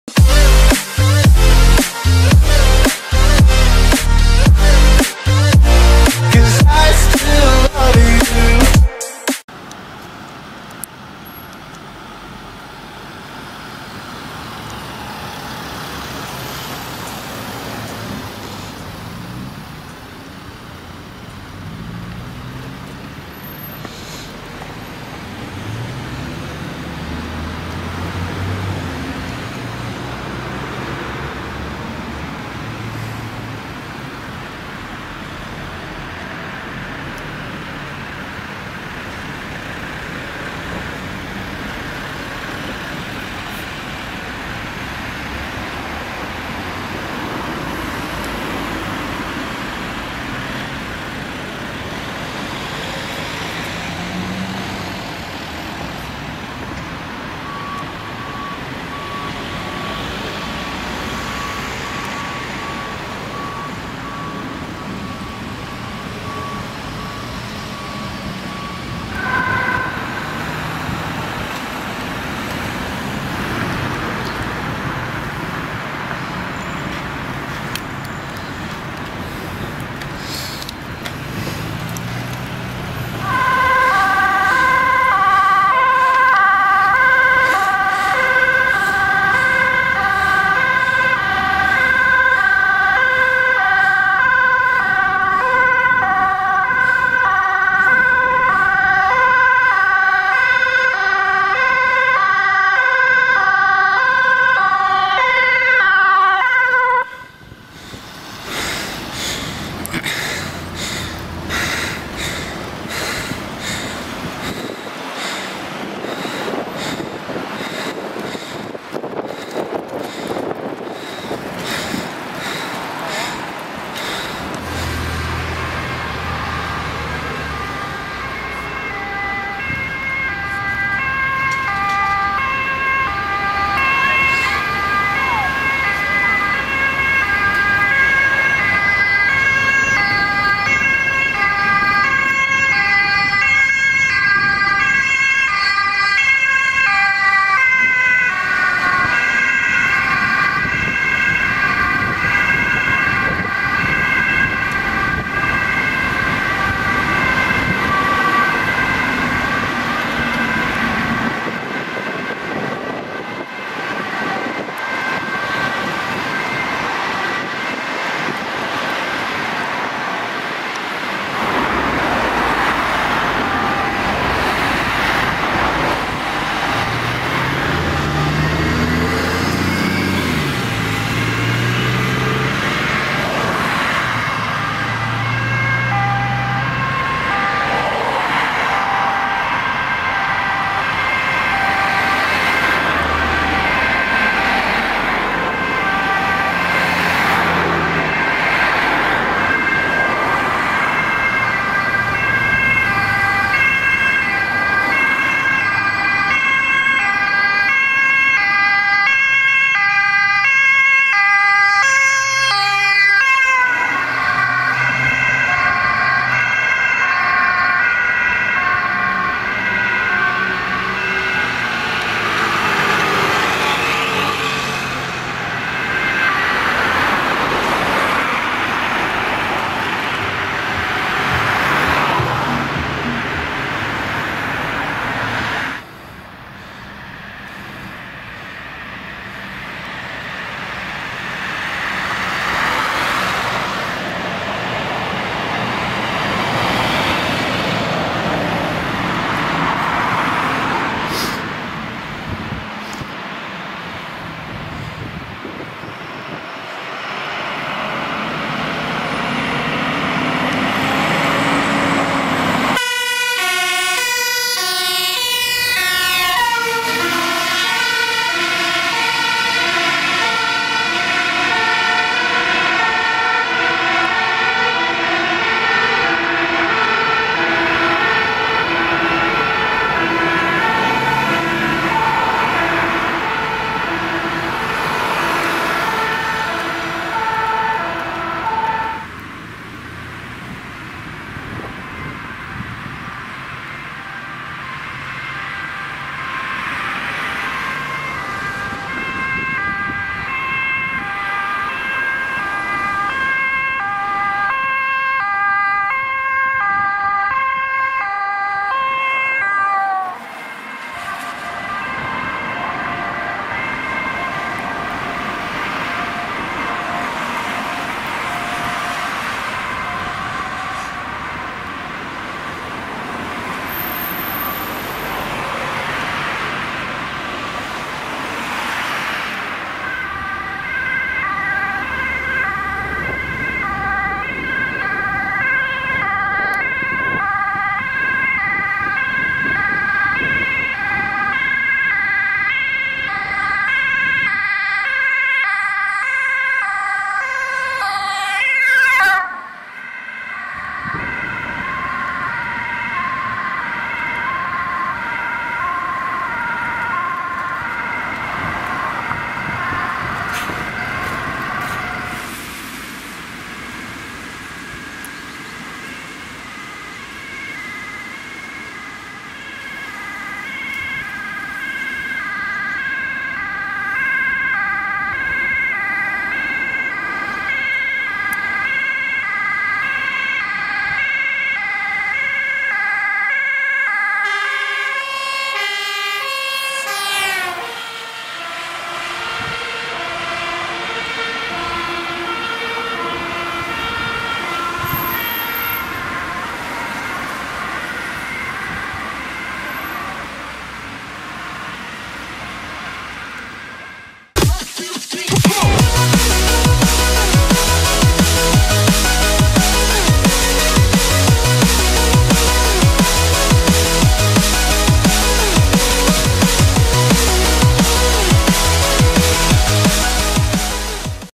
[Luchthoorn] Brandweer en Ambulance met spoed in Enschede naar en van verschillende meldingen
In deze video: Brandweer en Ambulance met spoed in Enschede naar en van verschillende meldingen

Voertuigen in deze video:
- MICU Ambulance 04-302
- Ambulance 05-103
- Ambulance 05-114
- Ambulance 05-115
- Ambulance 05-116
- Ambulance 06-159
- Brandweer Enschede Tankautospuit 05-4131

Deze video mag op geen enkele manier gebruikt worden zonder toestemming van de eigenaar, op alle video's rust copyright.